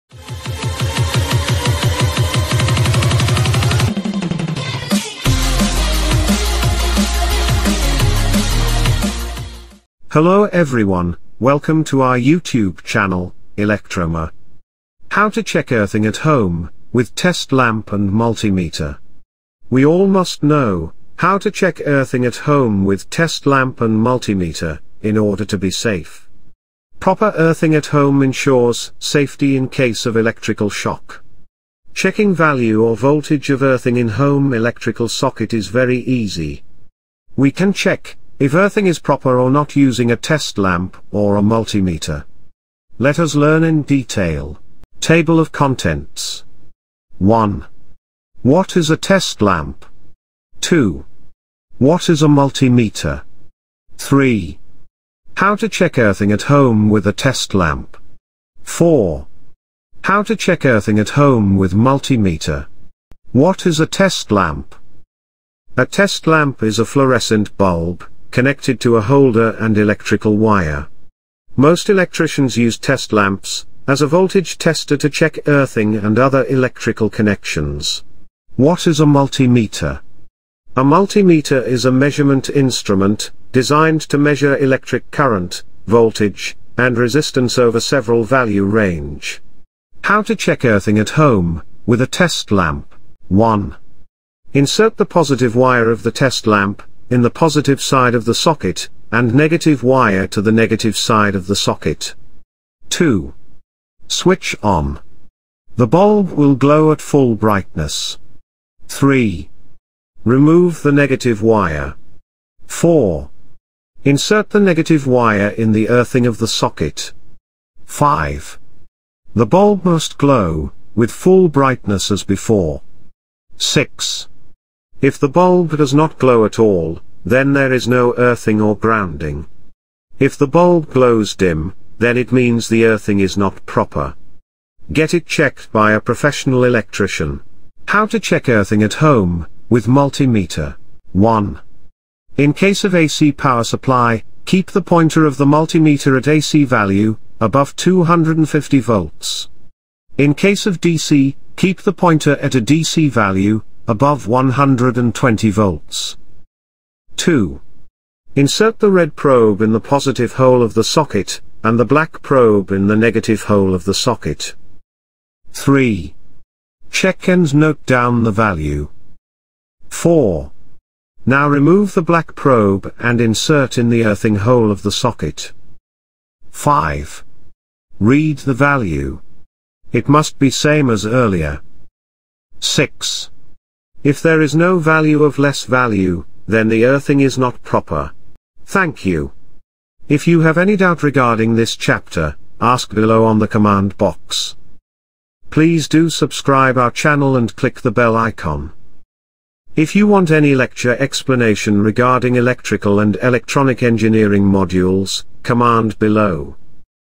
How to Check Earthing at Home with Test Lamp and Multimeter.
We all must know How to Check Earthing at Home with Test Lamp and Multimeter in order to be safe. Proper earthing at home ensures safety in case of electrical shock. Checking value / voltage of earthing in home electrical socket is very easy. We can check if earthing is proper or not using a Test Lamp or a Multimeter. Let us learn in detail.

1. What is a Test Lamp?
2. What is a Multimeter?
3. How to Check Earthing at Home with a Test Lamp.
4. How to Check Earthing at Home with Multimeter.